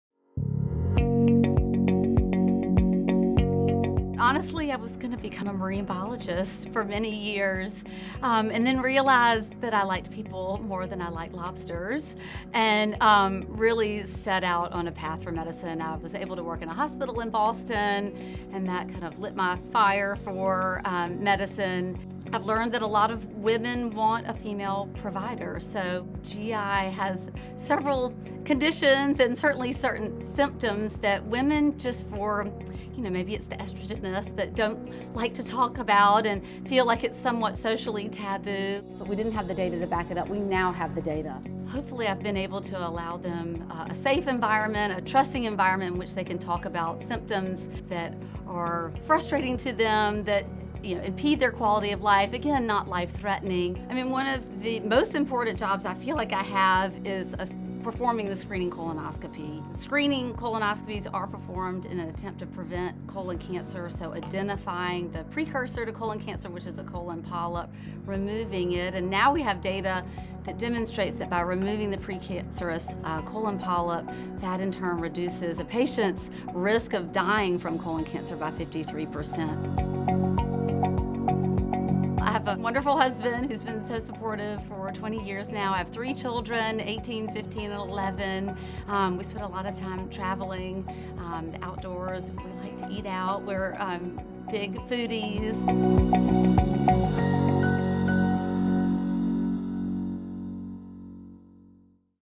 Elizabeth Lindsey, M.D. | Provider Spotlight
Elizabeth Lindsey, M.D., is a board-certified gastroenterologist at Williamson Medical Group with advanced training in diagnosing, managing and healing the gastrointestinal system.